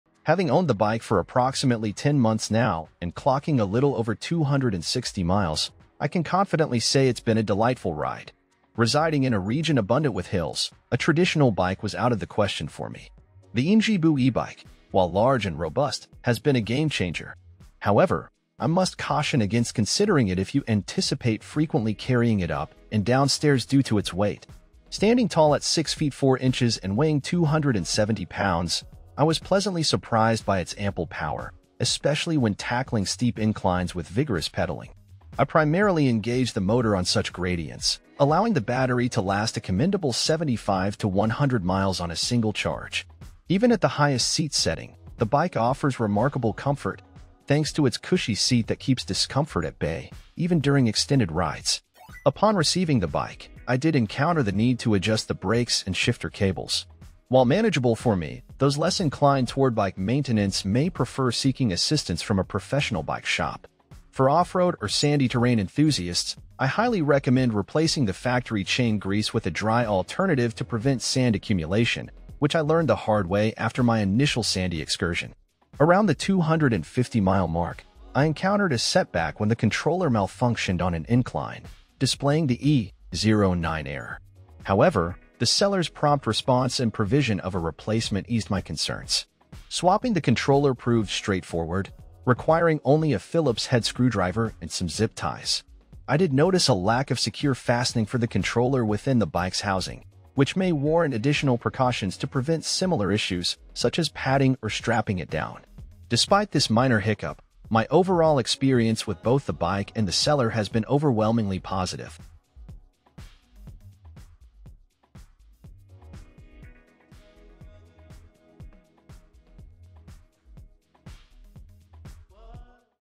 YinZhiBoo smlro xdc600 E-Bike Fat Tire: 10-Month Journey & 260 Miles
YinZhiBoo E-Bike Fat Tire

Top 5 Electric Bicycles:
Heybike Cityscape 2.0
Jetson Bolt
Jasion EB5
Oraimo Monster 100
Ancheer Mountain Bike

Experience the YinZhiBoo E-Bike like never before in our electrifying video review. Join us as we cover 10 months and 260 miles of thrilling rides, revealing its power, comfort, and essential maintenance tips. Don't miss out on this captivating journey - watch now!

00:00 Intro
00:24 Comfort
01:14 Controller Woes